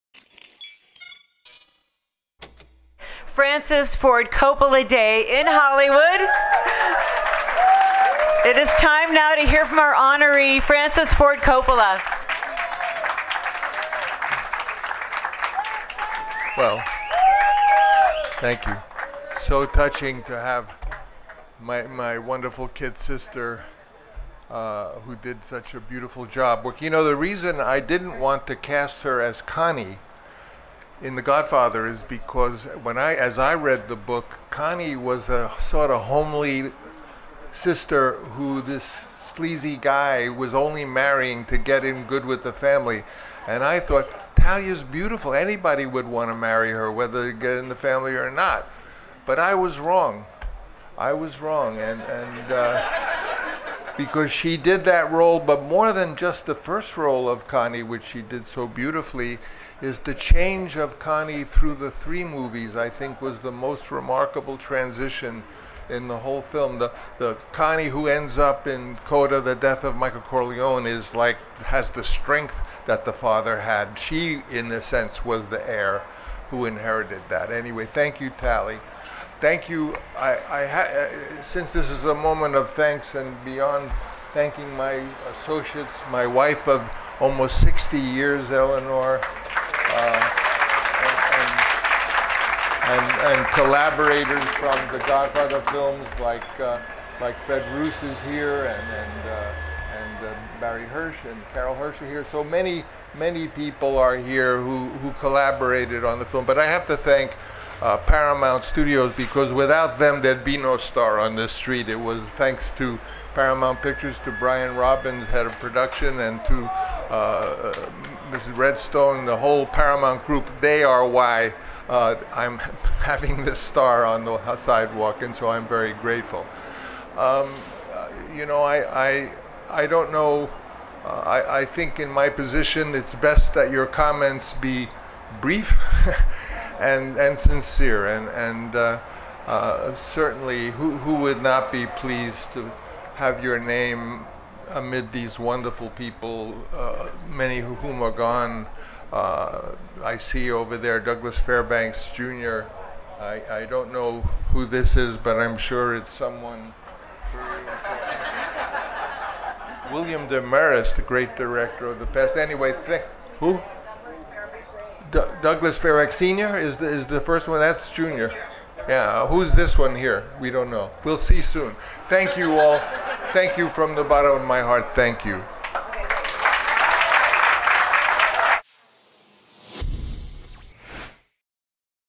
Francis Ford Coppola Speech at his Hollywood Walk of Fame Star Unveiling Ceremony
Film director Francis Ford Coppola speech at his Hollywood Walk of Fame Star unveiling ceremony held at the 6667 Hollywood Blvd in Los Angeles, California USA on March 21st, 2022